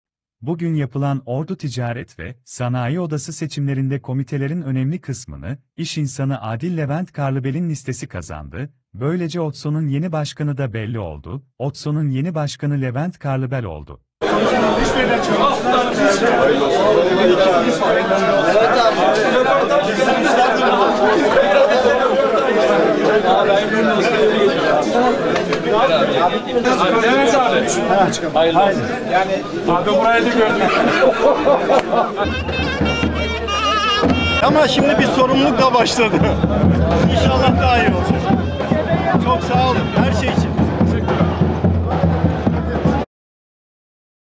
LEVENT KARLIBEL OTSO’NUN YENİ BAŞKANI OLDU
Bugün yapılan Ordu Ticaret ve Sanayi Odası seçimlerinde komitelerin önemli kısmını iş insanı Adil Levent Karlıbel’in listesi kazandı
Böylece OTSO’nun yeni başkanı da belli oldu.
OTSO’nun yeni başkanı Levent Karlıbel oldu.